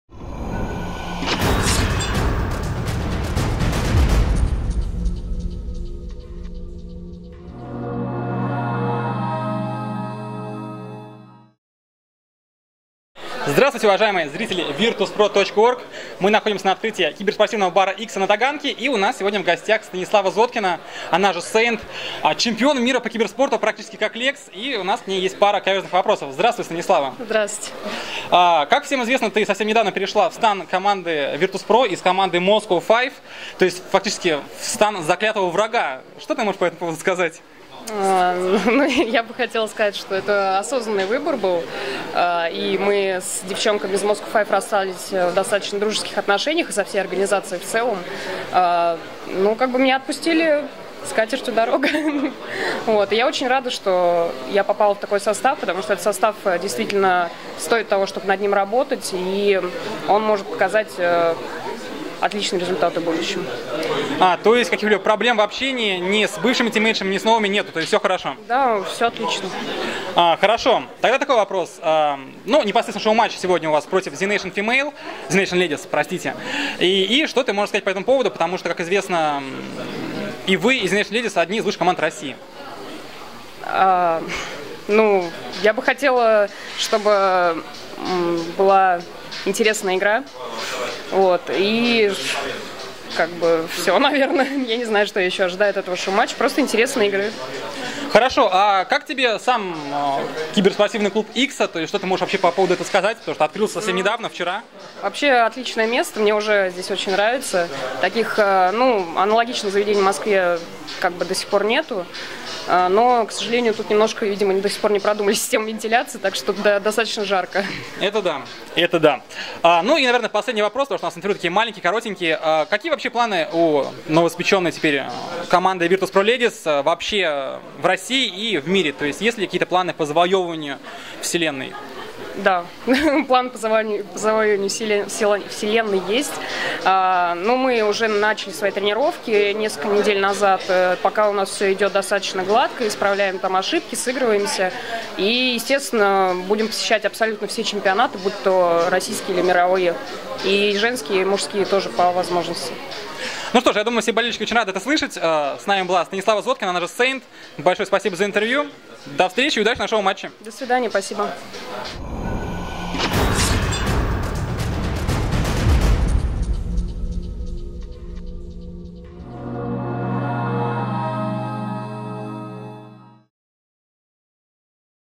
Интервью с Sa1nT
Интервью с игроком команды Virtus.pro Ladies, взятое на открытии киберспортивного бара «Икса».